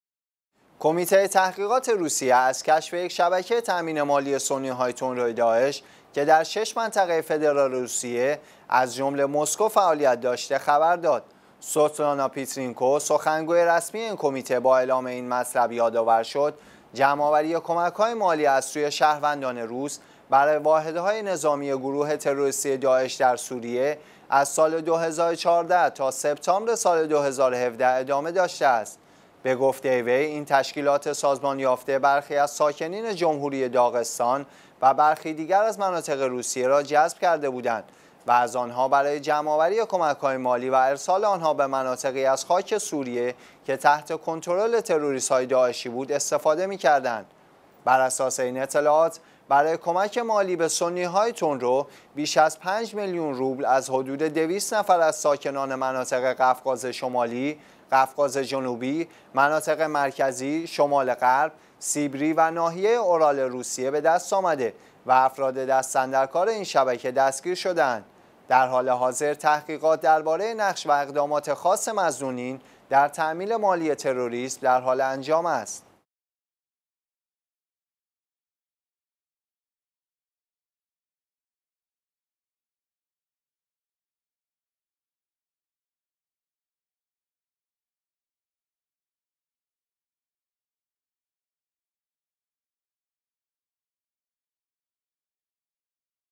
کشف شبکه بزرگ تامین مالی داعش در ۶ منطقه روسیه
کمیته تحقیقات روسیه از کشف یک شبکه تامین مالی سنی های تندروی داعش که در 6 منطقه فدرال روسیه، ازجمله مسکو، فعالیت داشته، خبر داد.
سوتلانا پیترینکو، سخنگوی رسمی این کمیته با اعلام این مطلب، یادآور شد: جمع آوری کمک‌های مالی از سوی شهروندان روس برای واحدهای نظامی گروه تروریستی داعش در سوریه، از سال 2014 تا سپتامبر سال 2017 ادامه داشته است.
به گفته وی، این تشکیلات سازمان یافته برخی از ساکنین جمهوری داغستان و برخی دیگر از مناطق روسیه را جذب کرده بودند و از آنها برای جمع‌آوری کمک‌های مالی و ارسال آنها به مناطقی از خاک سوریه که تحت کنترل تروریست‌های داعشی بود استفاده می‌کردند.
بر اساس این اطلاعات، برای کمک مالی به سنی های تندرو بیش از 5 میلیون روبل از حدود 200 نفر از ساکنان مناطق قفقاز شمالی، قفقاز جنوبی، مناطق مرکزی، شمال غرب، سیبری و ناحیه اورال روسیه به دست آمده و افراد دست‌‌اندرکار این شبکه دستگیر شده‌اند.
در حال حاضر تحقیقات درباره نقش و اقدامات خاص مظنونین در تامین مالی تروریسم در حال انجام است.